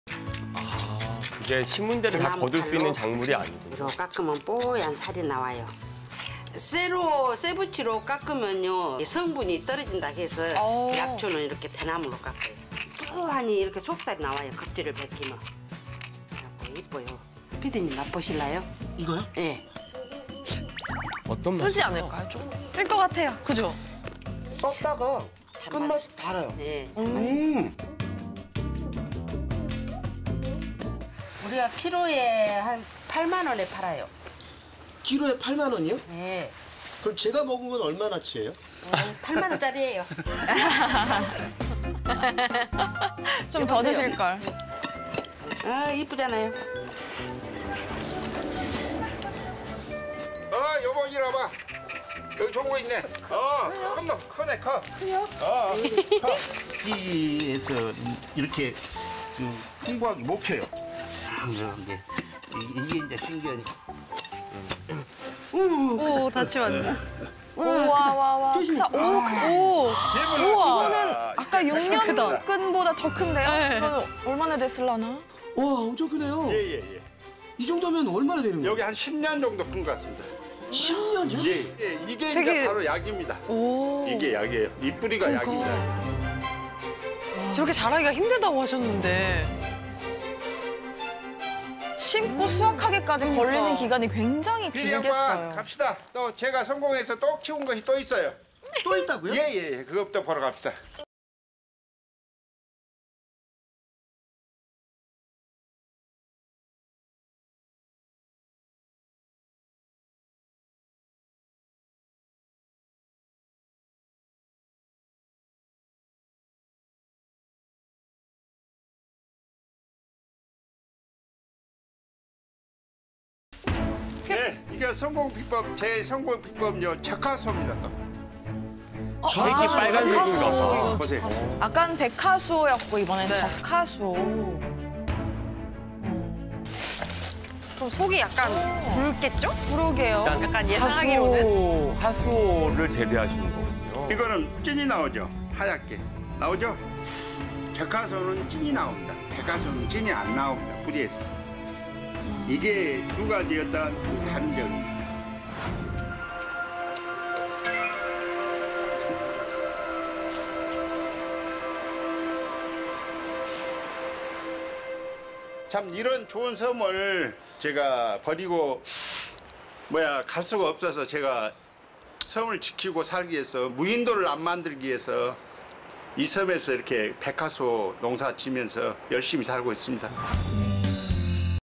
백하수오와 적하수오의 차이점
중국이 원산지인 적하수오는 독이 있어
반드시 법제 과정을 거치고 먹어야 한다.